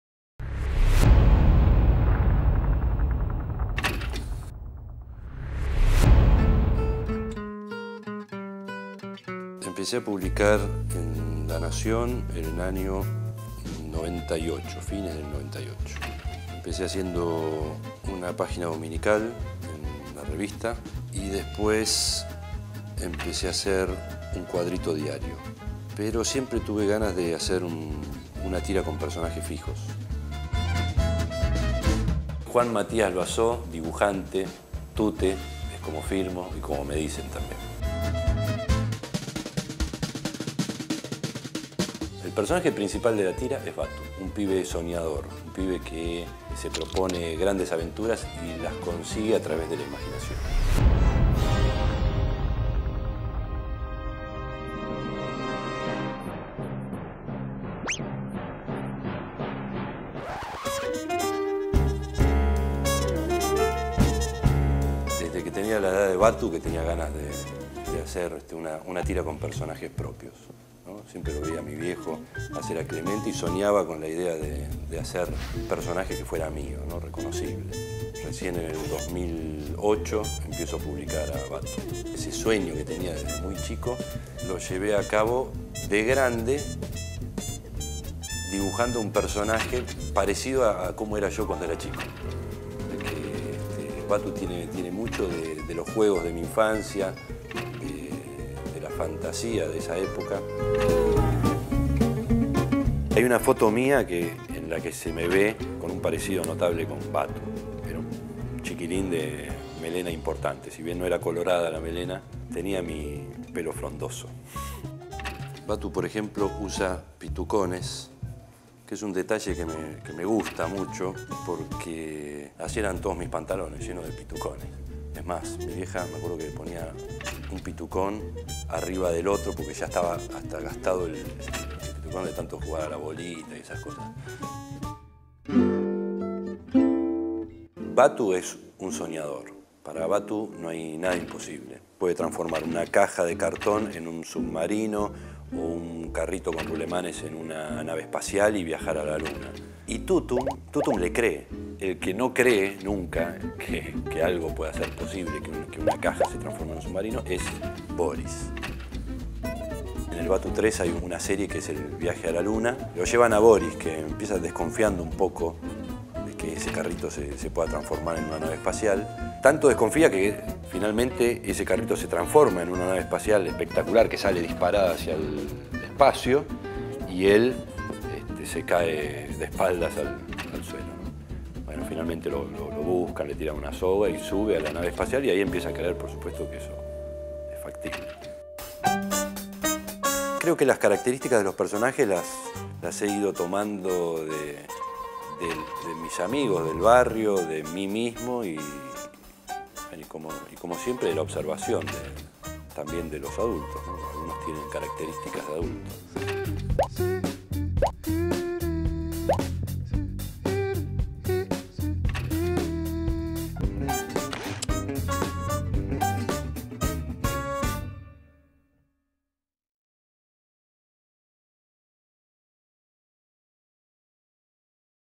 Book trailer de Batu 3
El tercer libro recopilatorio de la tira "Batu" que Tute publica desde hace tres años en el diario La Nación, suplemento Espectáculos.

En la línea del mejor Quino, Tute desarrolla la historia de Batu, Tutum, Boris y Dafne. La idea es seguir apostando a este autor en toda su obra. El primer libro que publicamos del autor fue "Tute", su libro de cuadros; el segundo fue "Tute de bolsillo" que ya va por su tercera edición, el tercero, "Araca, corazón" también fue publicado en Debolsillo. Luego Batu y su continuación, que cada vez tiene más seguidores.

Recomiendo a sus lectores seguirlo en Facebook, en su propio blog: tuteblog.blogspot.com y en su página